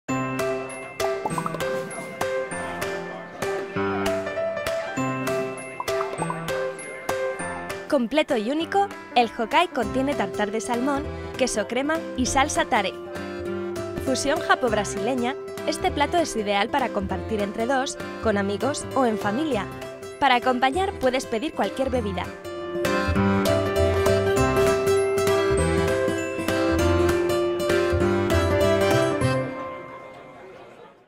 Restaurante Ikibana Parallel Barcelona | Hokkai | Check Guide
Completo y único, el Hokkai contiene tartar de salmón, queso crema y salsa tare. Fusión Japo Brasileña, este plato es ideal para compartir entre dos, con amigos o en familia. Para acompañar puedes pedir cualquier bebida.